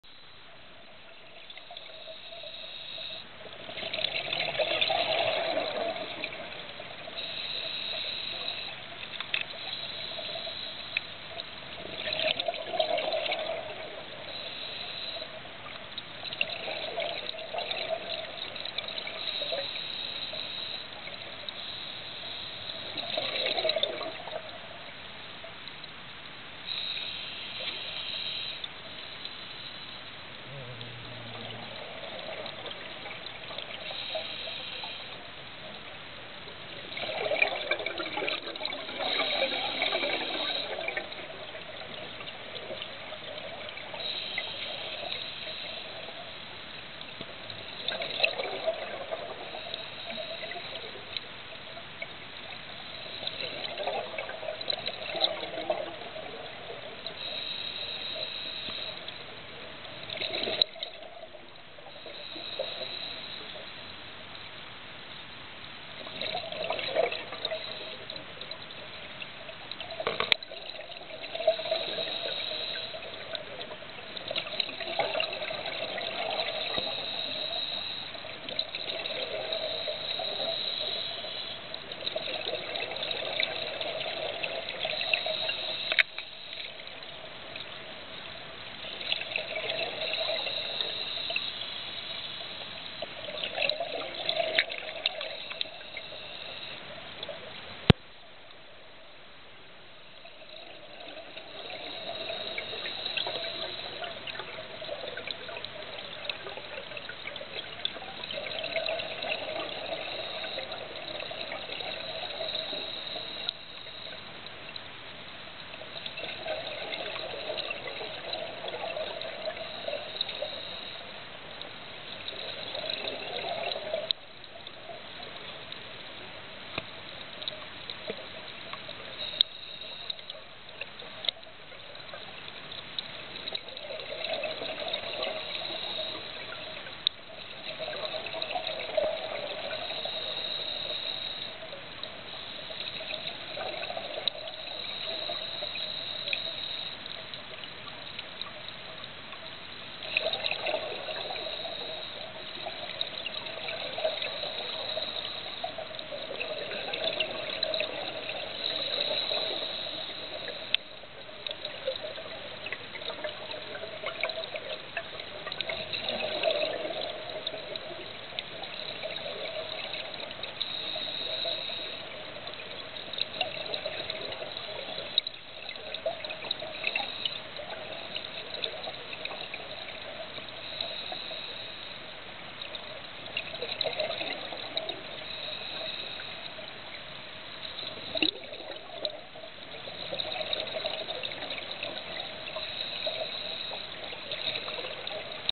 南紀白浜沈☆４・１９
今日の白浜は一日曇空でしたが海の中は透明度も一気にUPしており沈船では１５ｍ近くは見えていましたよぉ～「キンメモドキ」の大群で今日も癒されてきました（●＾o＾●）